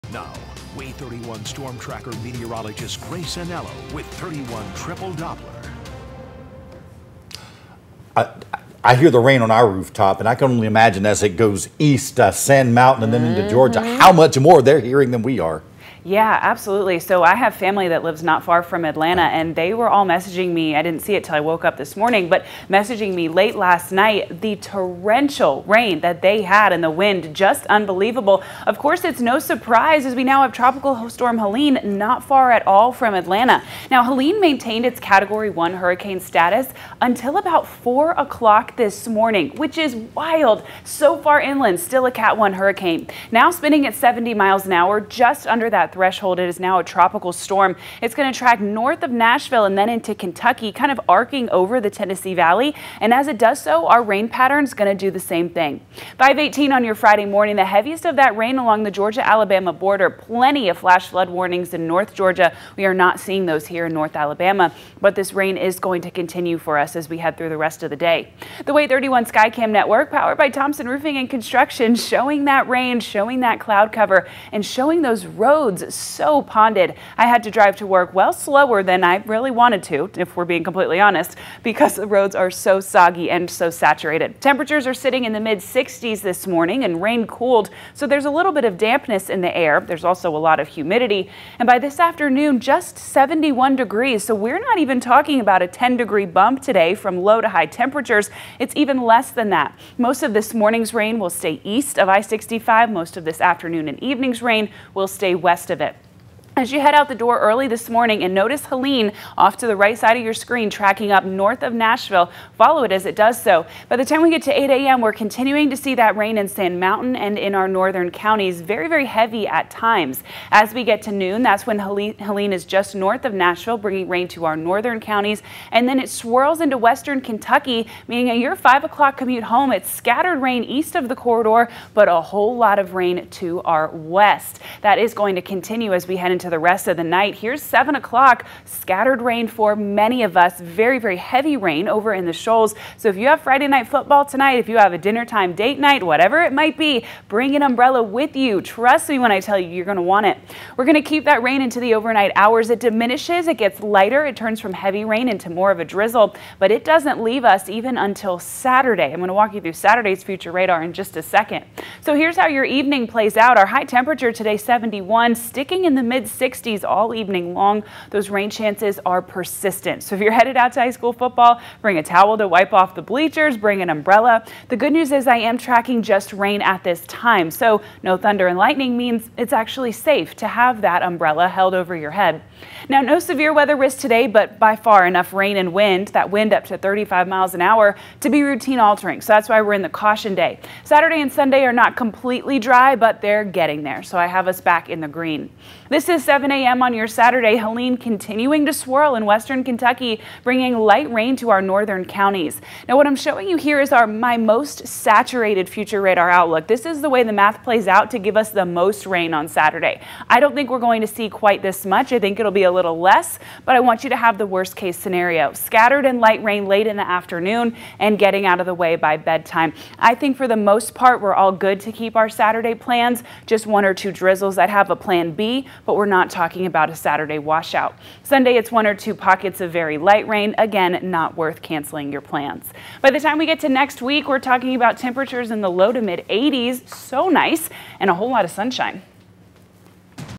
Grace Anello's Friday Morning Forecast 9.27.24
Grace is continuing to track Tropical Storm Helene as it brings more rain to the Tennessee Valley